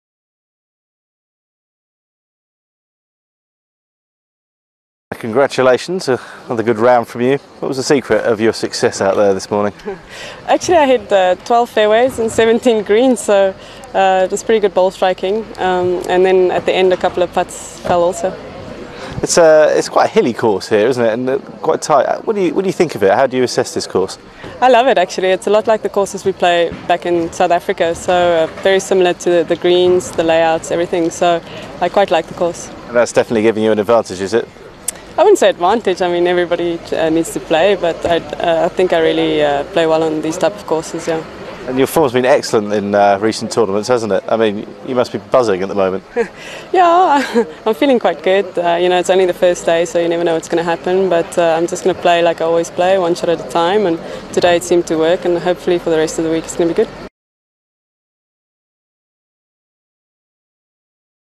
Open de Espana Pace interview
Lee-Anne Pace gives her thoughts from the Open de Espana.